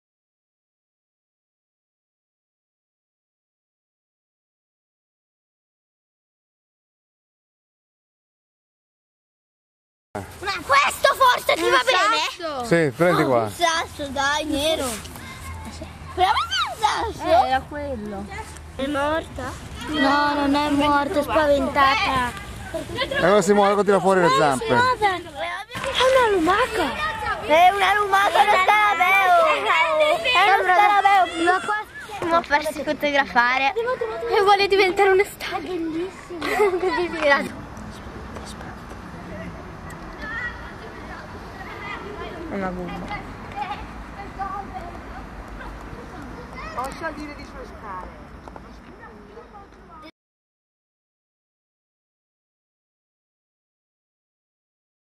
Chrysomelidae
Scenes from old videos of children, looking for insects in their school Garden, Brescia (Italy, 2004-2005)